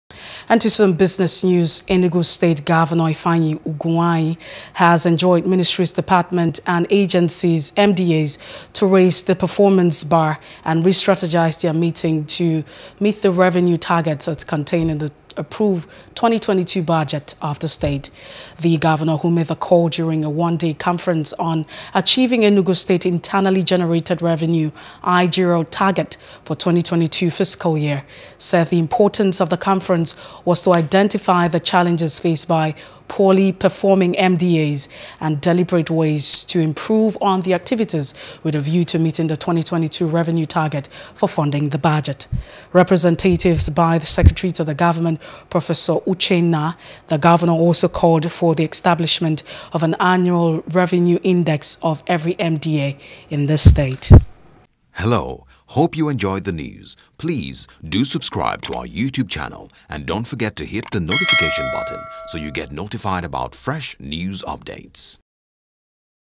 Budget Funding: Ugwuanyi Tasks M.D.A.S. On Meeting Revenue Targets | BUSINESS
Enugu State Governor, Ifeanyi Ugwuanyi has enjoined Ministries, Departments, and Agencies (M.D.A.s) to raise their performance bar and re-strategize on meeting their revenue targets as contained in the approved 2022 budget of the state.